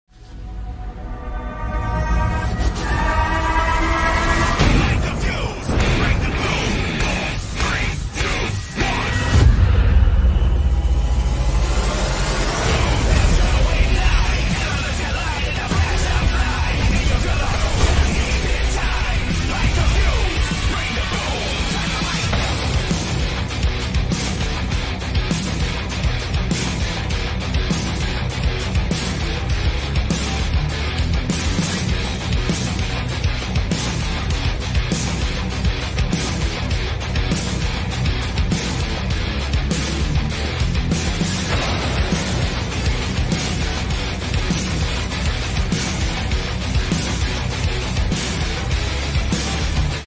AEW Dynamite;3 Year Anniversary Show Review
Dynamite celebrates its 3rd Anniversary where it all began;Washington D.C